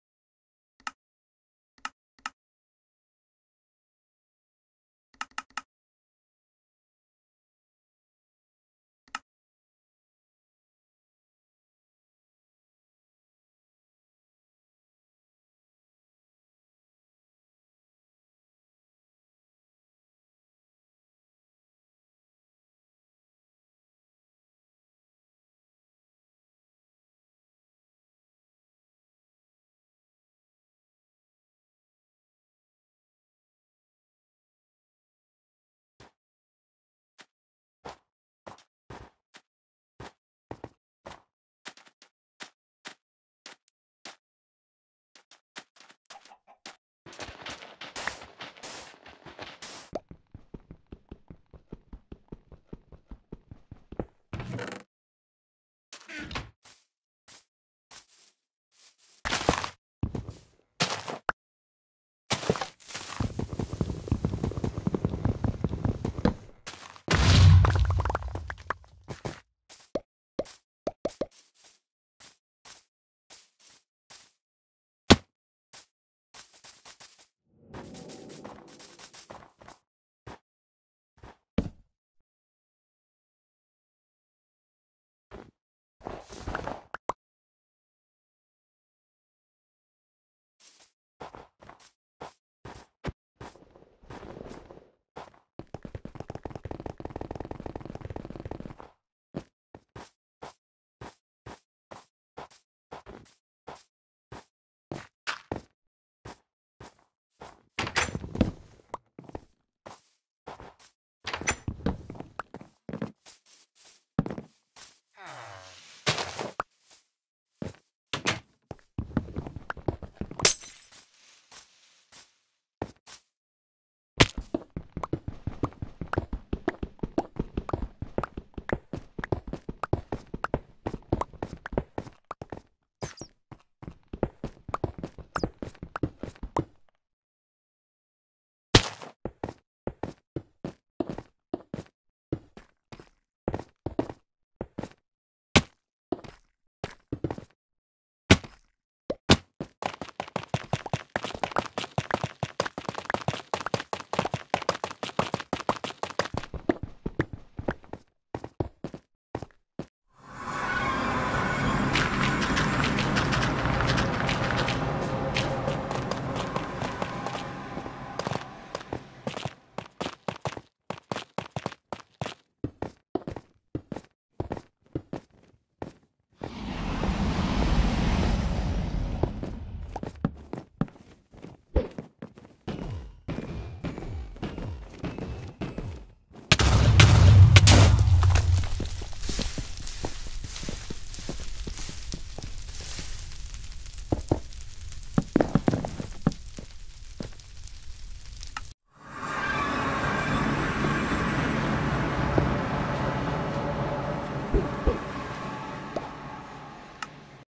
2:43 ssp pb
The amount of mistakes 1:00 ice boat 1:29 first stone mined 2:05 ee the 38 second end split was simply the cherry on top i will definitely improve trust me also 2 runs before this i hit rng cycle lol